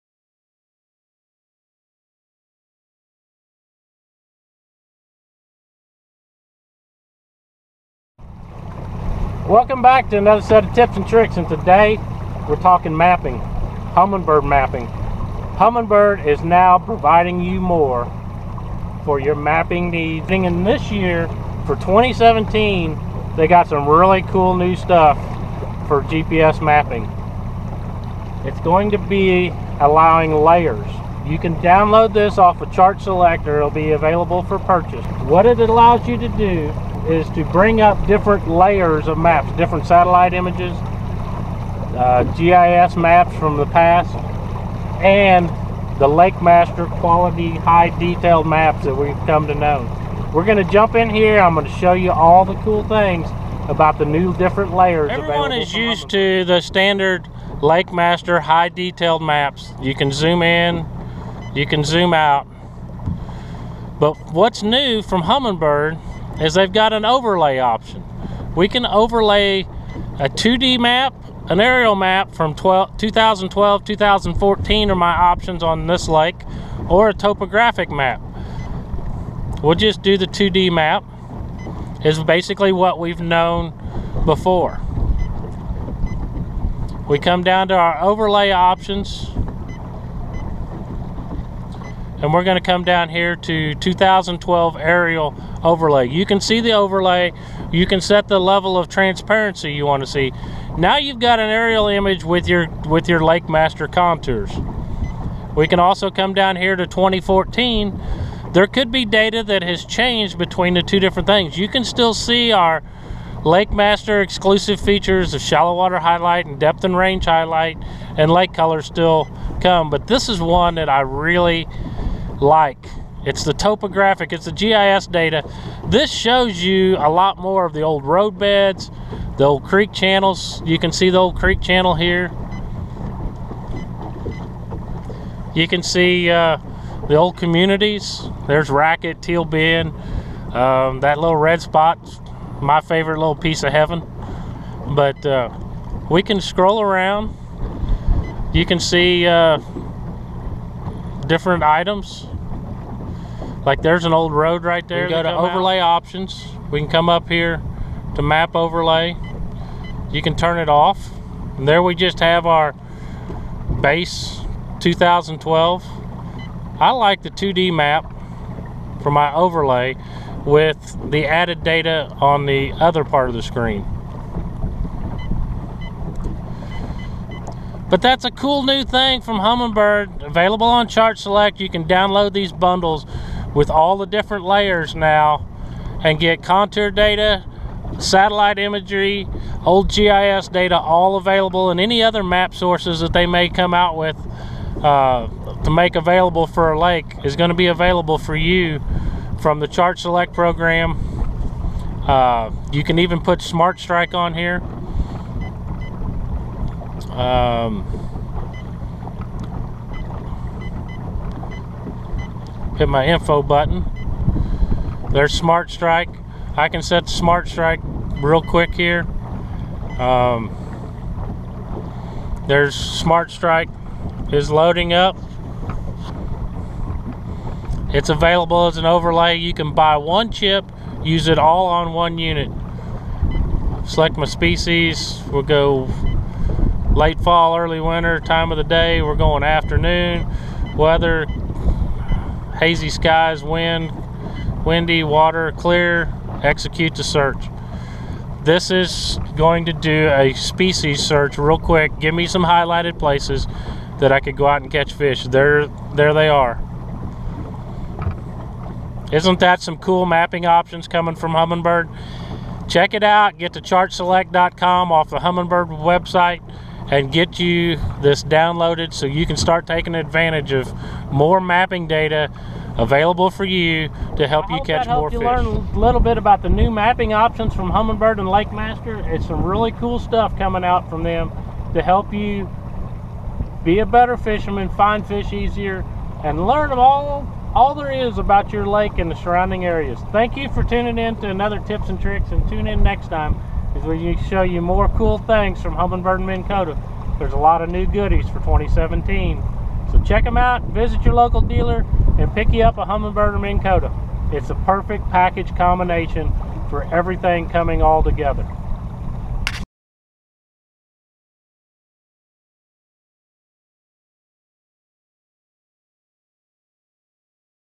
Tips 'N Tricks 185: Humminbird HELIX Chart Layers (NEW for 2017)
NEW for 2017 is Humminbird Chart Layers for added mapping choices...available via ChartSelect. Now Includes Aerial Options, GIS Data, SmartStrike and LakeMaster Super High Def Mapping all viewable as a Chart Download Package. Get More...Get a Humminbird